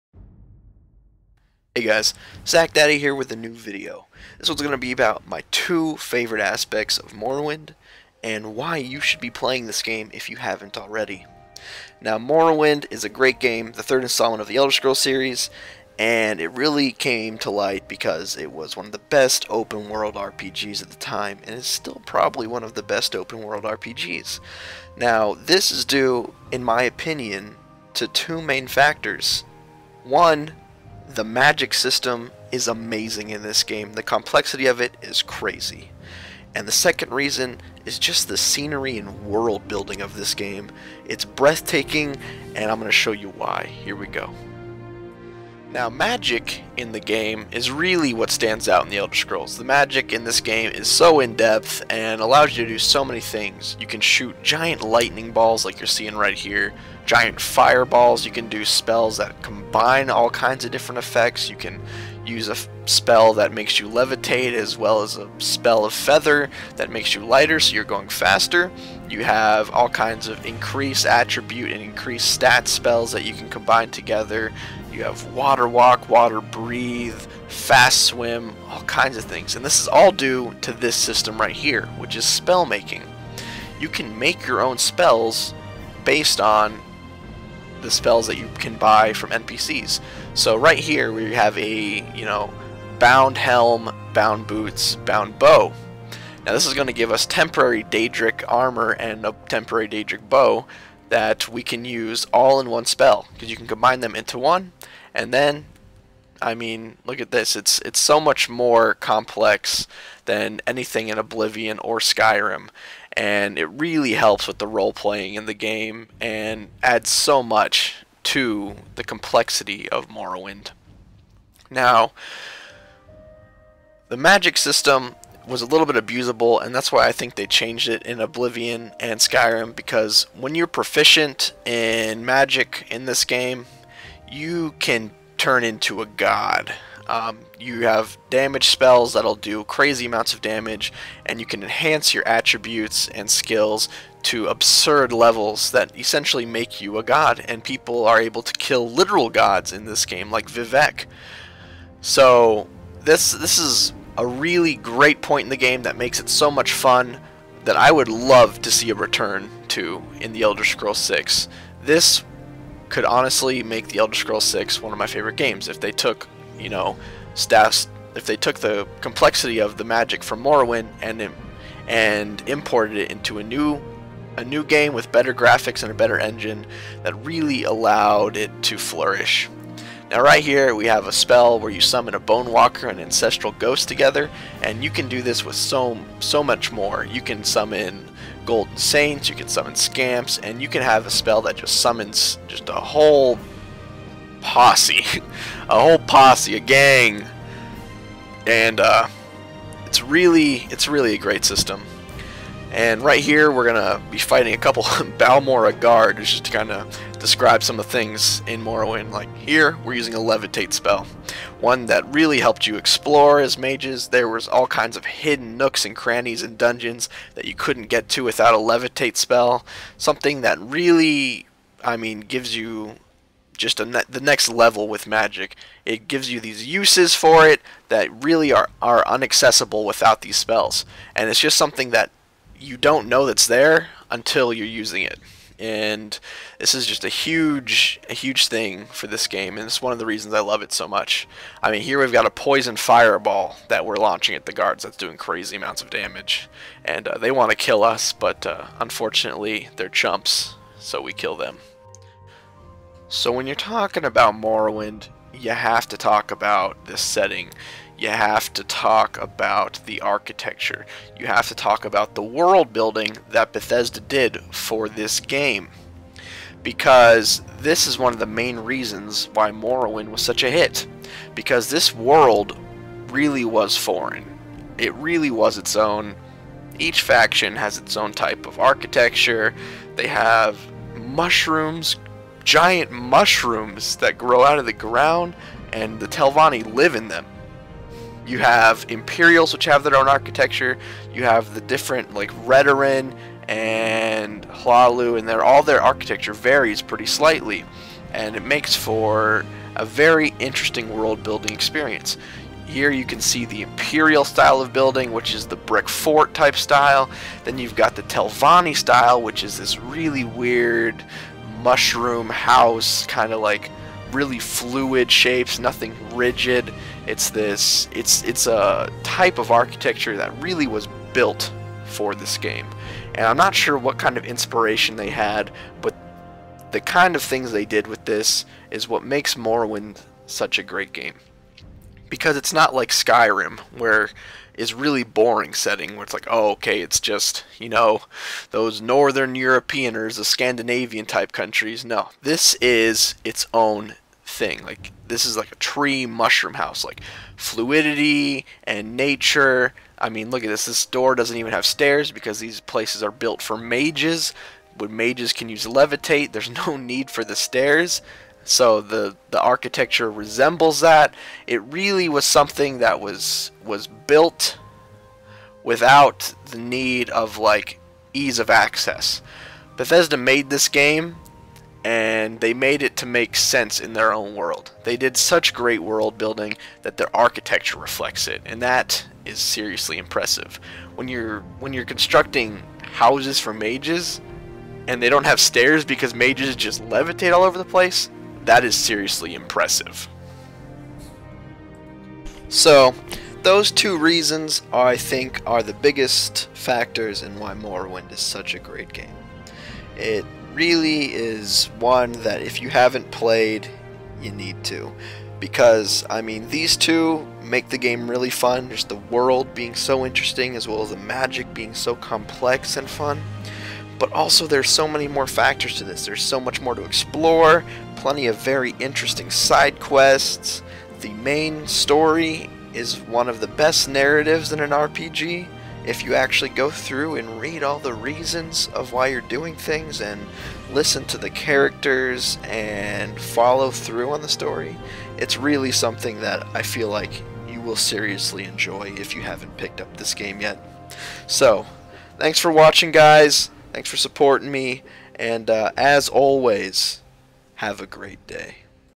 The Two Main Reasons You Should Play Morrowind
Visit My New Site Where I Review Morrowind! Bucknakedgaming.com
My two favorite reasons why Morrowind should be on your must play list.